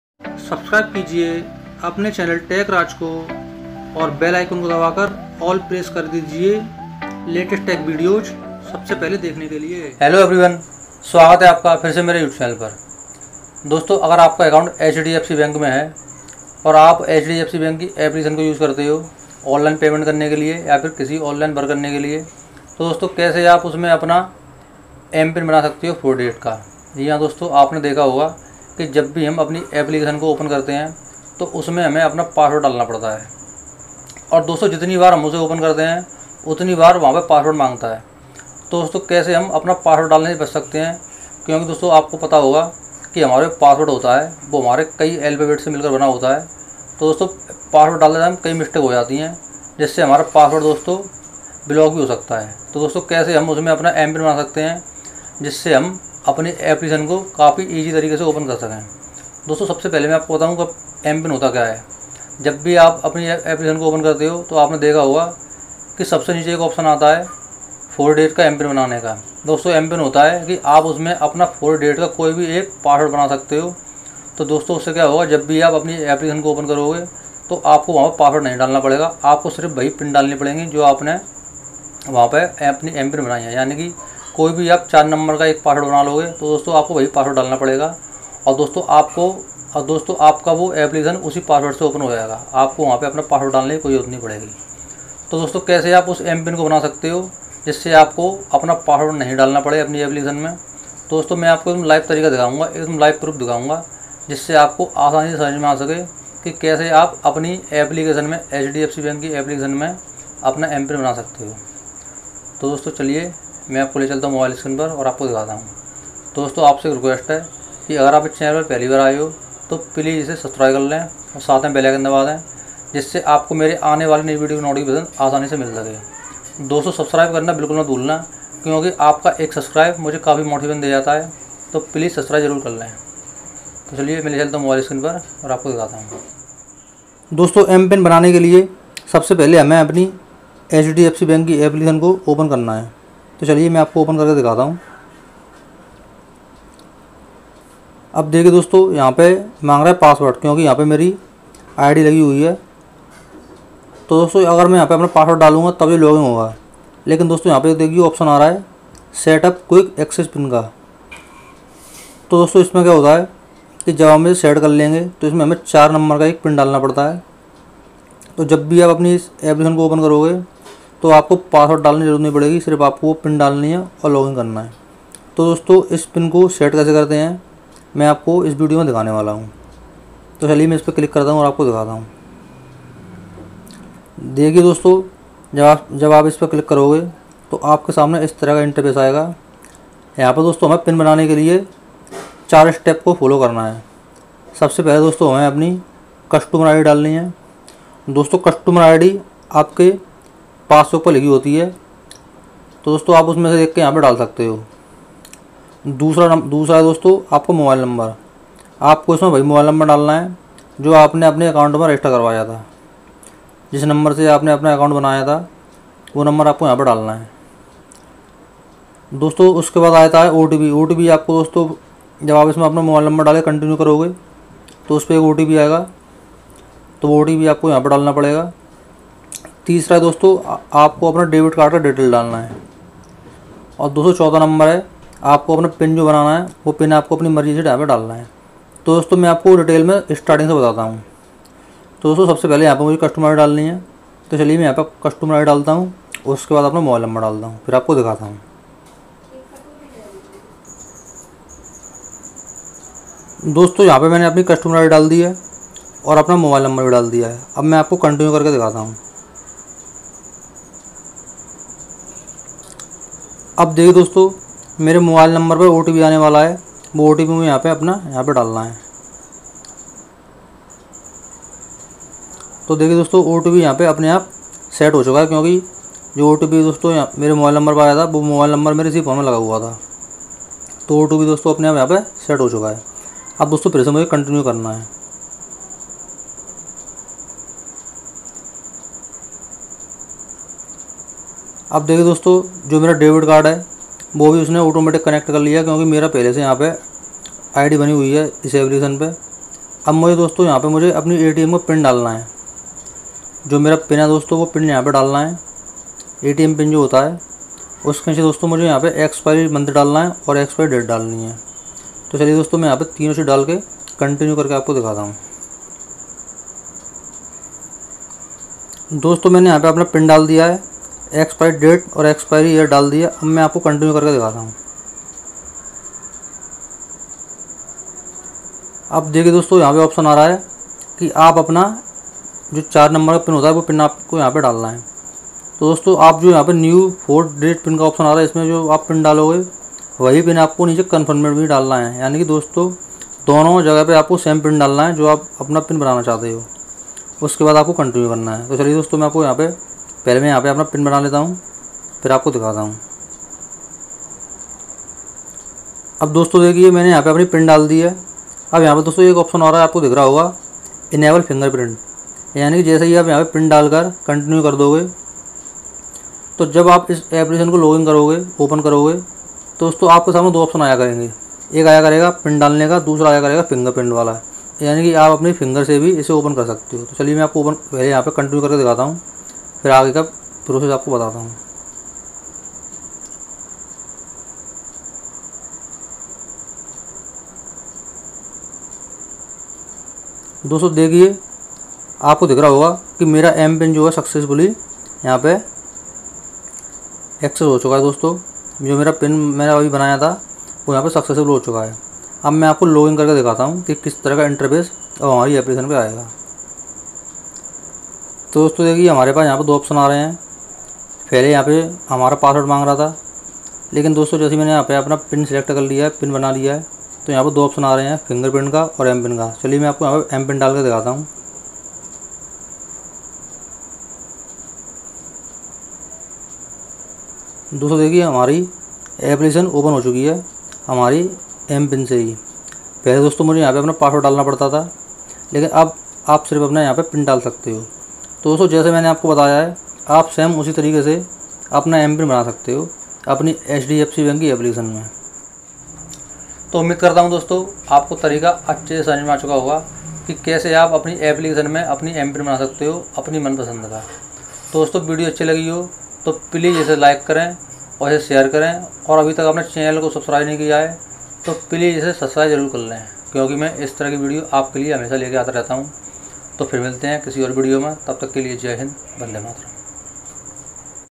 Hdfc bank m pin generation in 2020, eazy method
Dosto es video me maine hdfc bank ki m pin generate krne ka tarika bataya h. Jisse apko app opn krte tym apna pswrd na dalna pde kyuki dosto m pin se app jldi opn ho jati h or pswrd me kabhi kabhi wrng typ ho jata h to app opn nhi ho pati